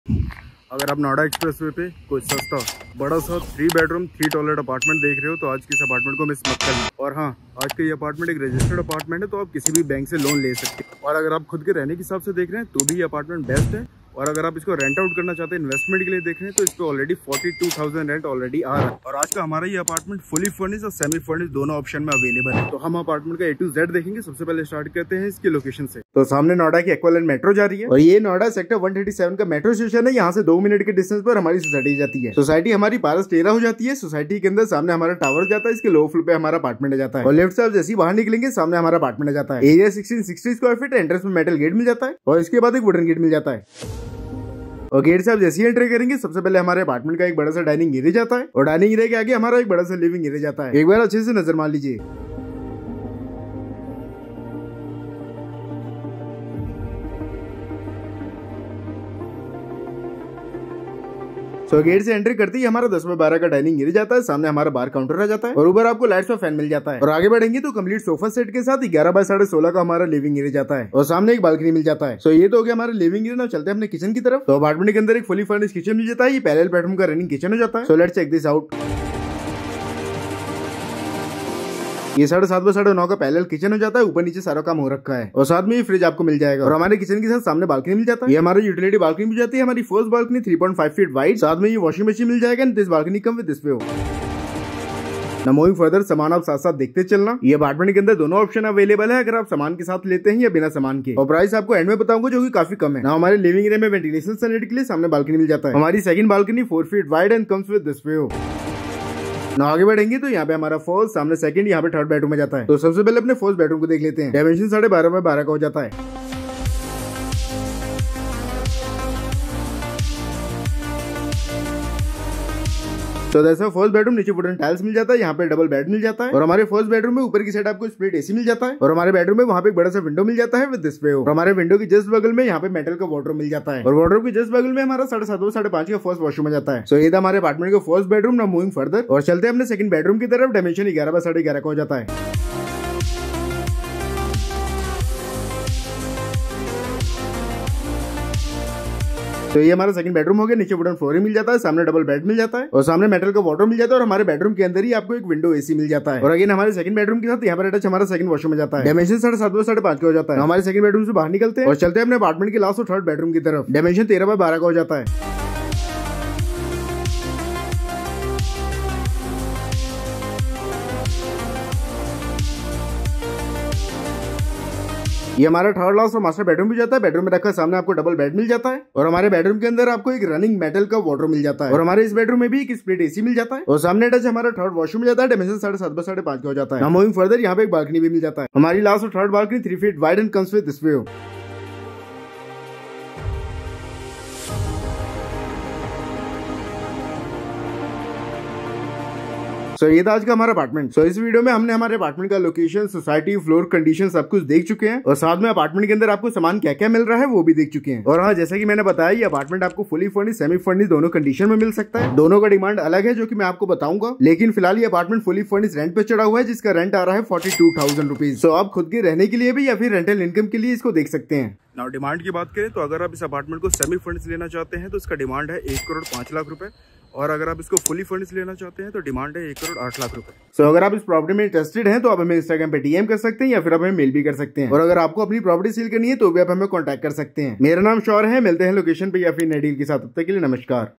Noida में इतना बड़ा Fully Furnished 3BHK Society का Apartment इतने कम दाम में नहीं मिलेगा || Saurya
Hi Saurya Here. A big fully furnished 3BHK Apartment is available for sale in Society On Noida Expressway. This Apartment comes with amazing rental income too. So if you are looking for investment or self use big 3bhk Apartment in Noida at a very affordable price, don't forget to check this big fully furnished 3BHK Society Flat of Noida.

Here Is Property Details
Location - Sector-137, Noida
Society - Paras Tierea
Layout - 3BHK
Floor - Lower
Size - 1660sqft
Parking - One Reserved Car Parking
Condition - Fully Furnished & Semi Furnished